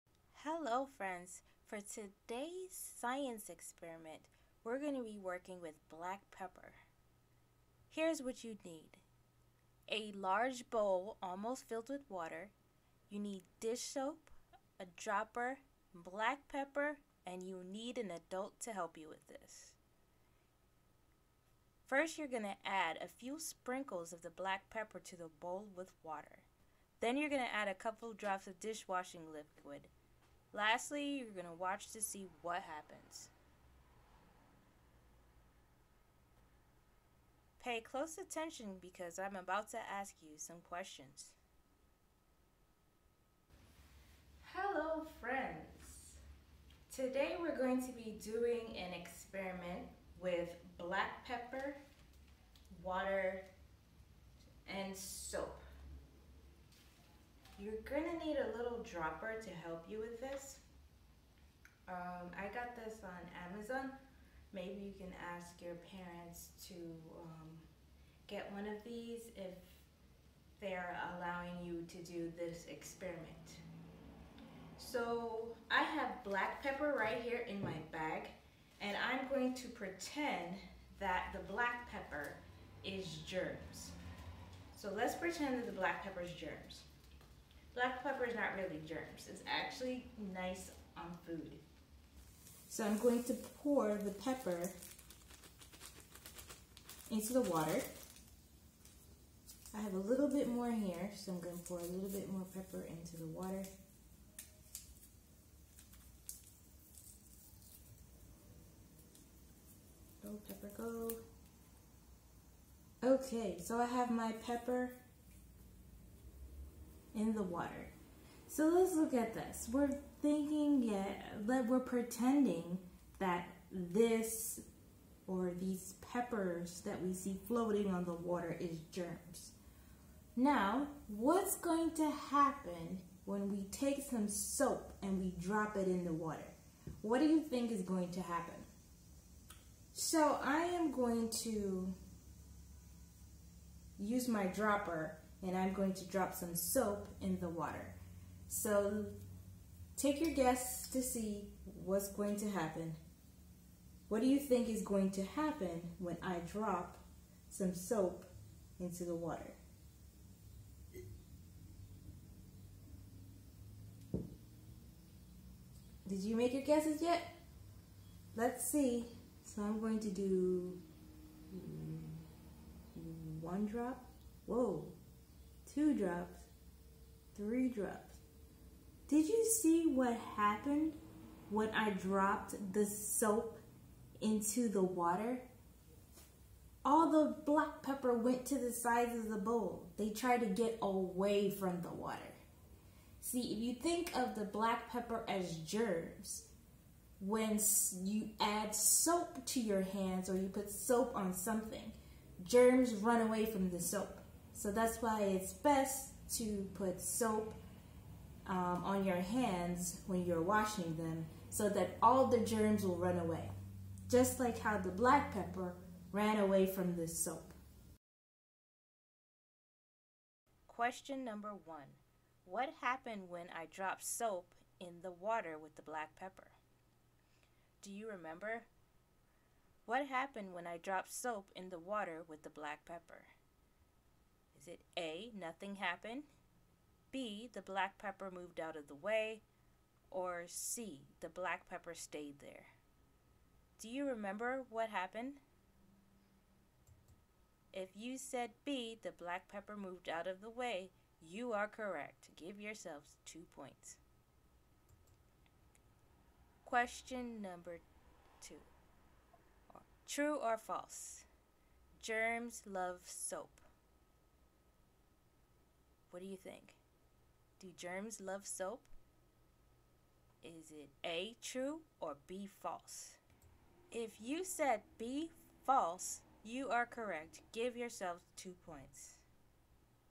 Black Pepper Experiment
This experiment is great to teach students how germs react to soap. You need black pepper, water and dish soap. I demonstrate what to do in this video.

If you like this video, please click the thumbs up, comment, and share.